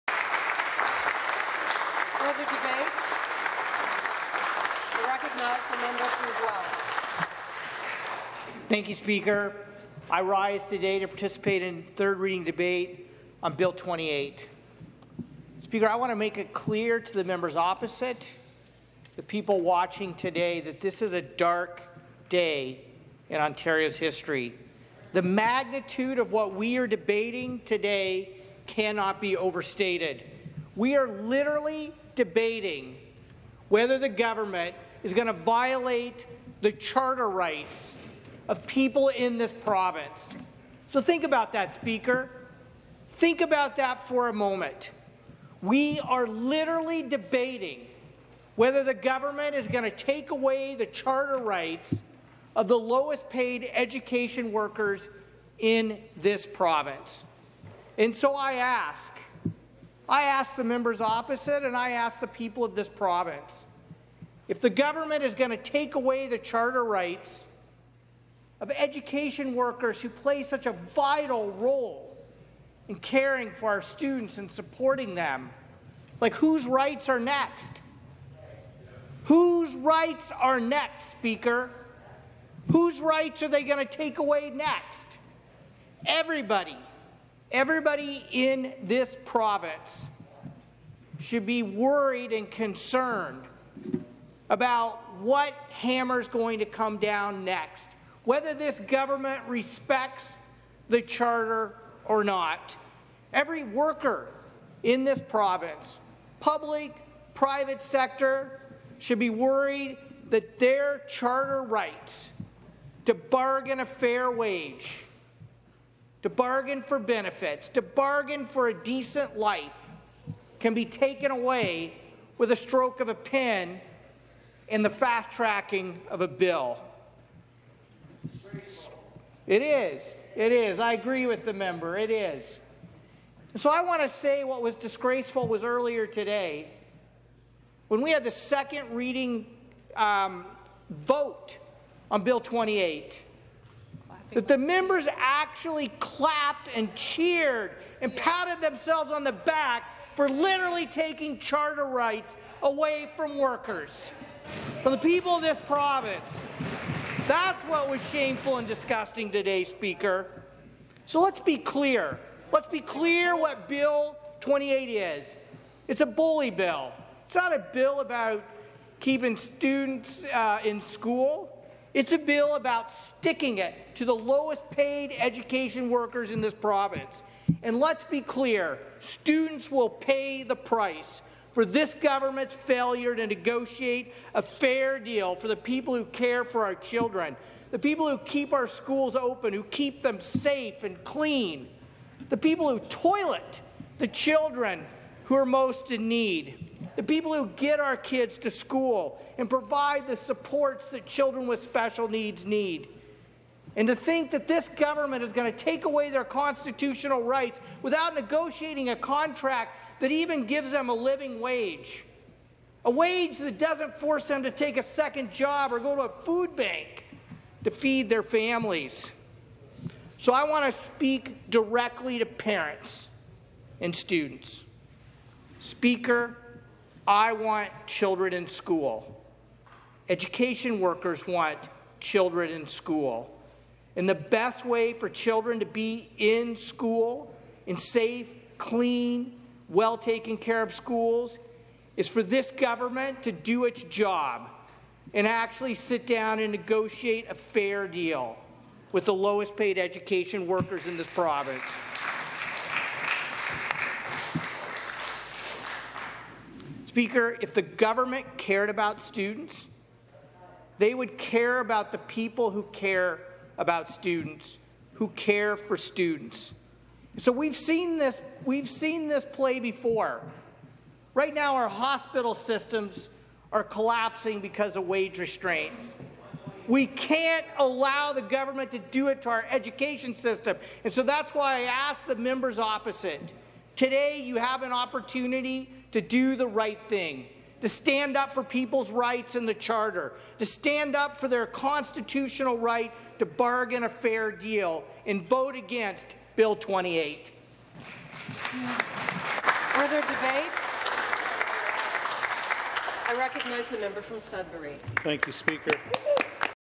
Bill 28 strips Ontario's education workers of their rights | Mike Schreiner debates
The use of the notwithstanding clause to strip away workers’ collective bargaining rights was never justified and could only end one way – with schools closed and students caught in the middle.

Now Ford must move quickly to minimize the damage and get students back in class with fair bargaining. Bill 28 is bad legislation and it must be repealed – the sooner the better.